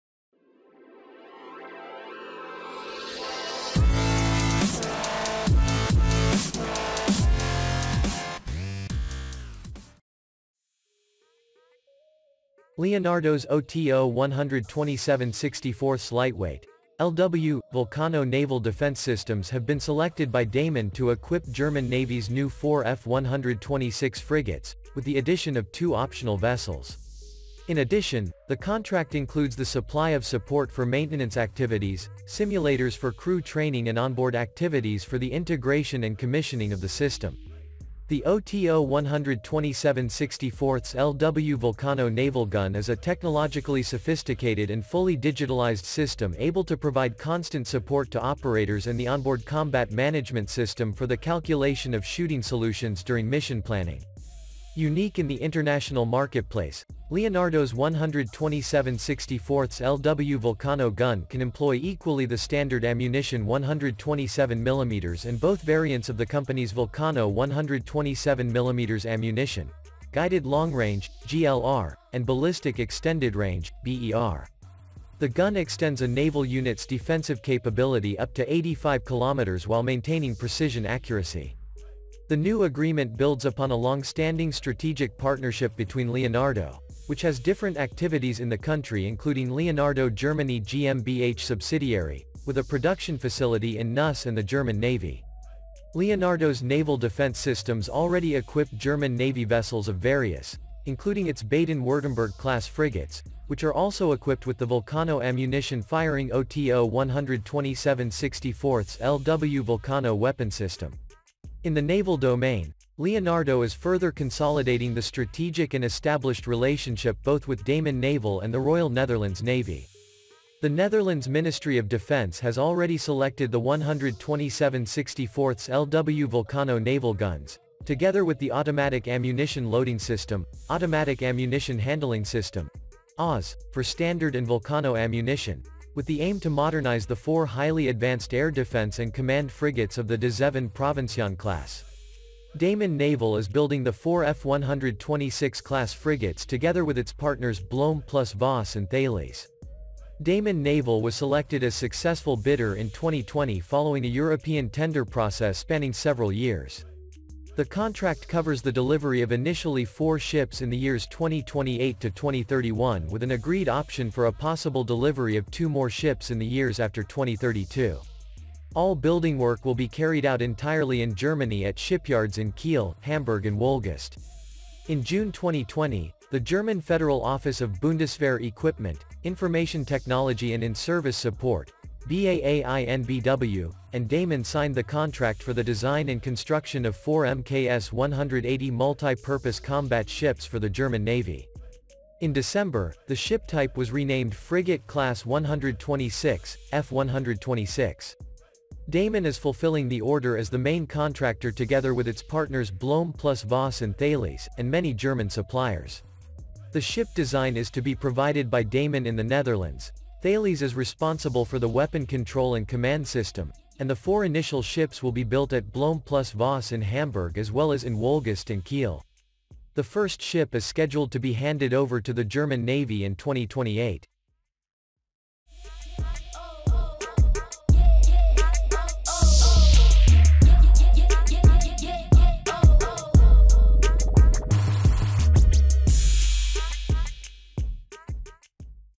The German Navy's four new F126 frigates will be armed with the Leonardo 127mm Vulcano Gun!
Leonardo's OTO 127/64 LightWeight (LW) Vulcano naval defence systems have been selected by Damen to equip German Navy’s new four F126 frigates, with the addition of two optional vessels.
In addition, the contract includes the supply of support for maintenance activities, simulators for crew training and on-board activities for the integration and commissioning of the system.
The OTO 127/64 LW Vulcano naval gun is a technologically sophisticated and fully digitalized system able to provide constant support to operators and the on-board Combat Management System for the calculation of shooting solutions during mission planning. Unique in the international marketplace, Leonardo’s 127/64 LW Vulcano gun can employ equally the standard ammunition 127mm and both variants of the company’s Vulcano 127mm ammunition: Guided Long Range (GLR) and Ballistic Extended Range (BER). The gun extends a naval unit’s defensive capability up to 85 km while maintaining precision accuracy.